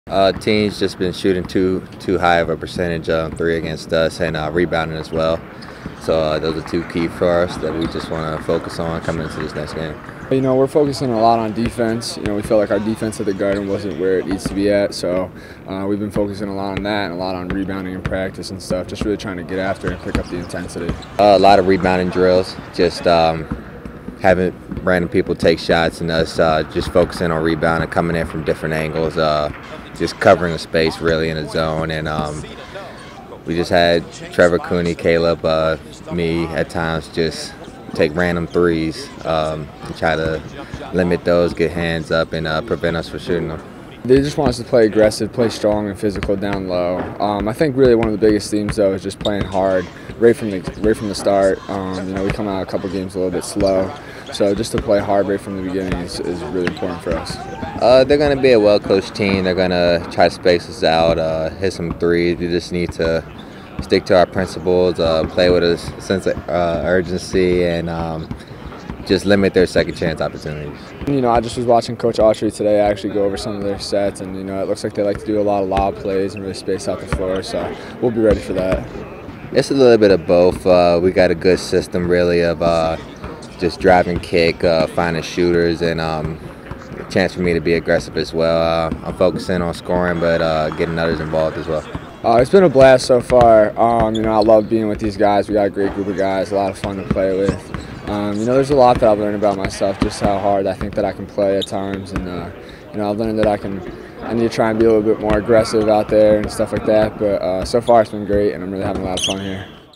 Preview | Syracuse vs. Cornell
Syracuse men's basketball graduate student guard Michael Gbinije and freshman forward Tyler Lydon discuss what the Orange have been working on in practice this week as they prepare to host Cornell at the Carrier Dome on Saturday at noon.